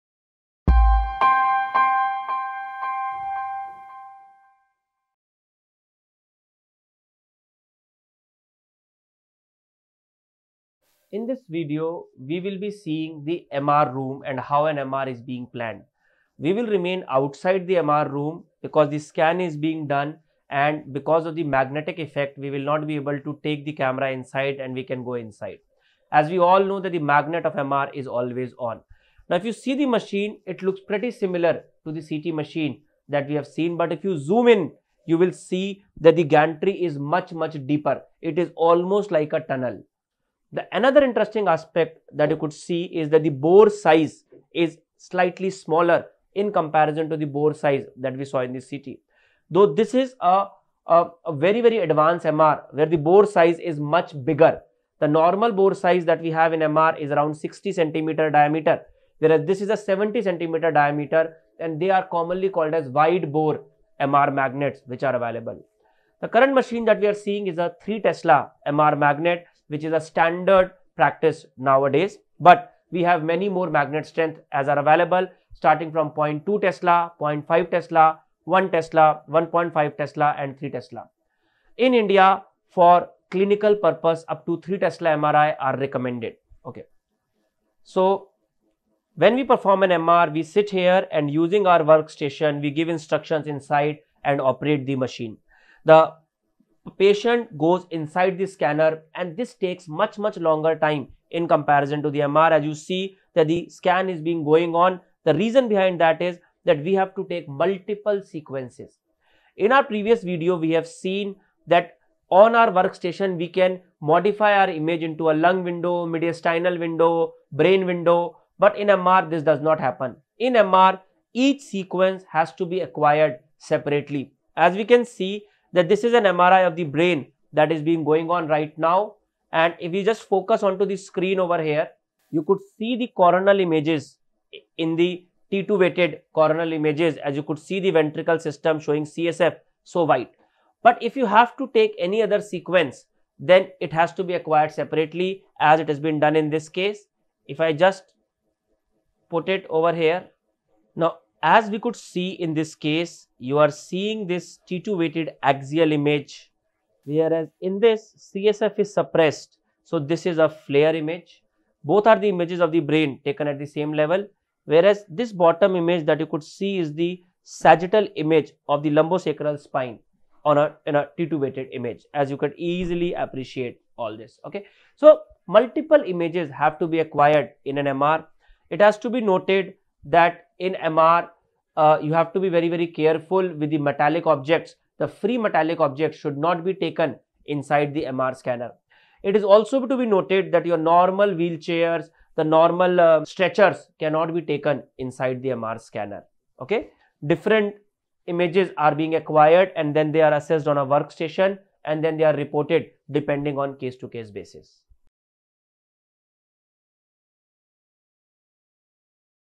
How is MRI planned? Clinical Wednesday - Dr. Rajat Jain
We are back with another clinical video for you all. Watch Dr. Rajat Jain explaining you all about MRI, how to plan MRI and more.

Available on both Android and iOS